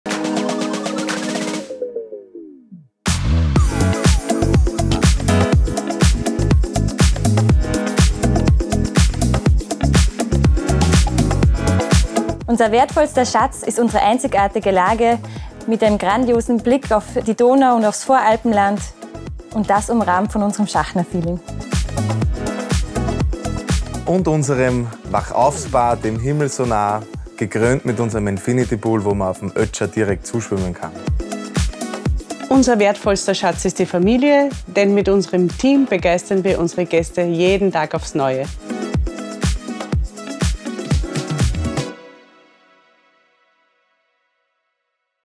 Hotel Schachner: WACHAUF mit allen Sinnen
Wachauf-Spa, Sky-Lounge und Restaurant bieten einzigartigen Blick bis zu 300 km.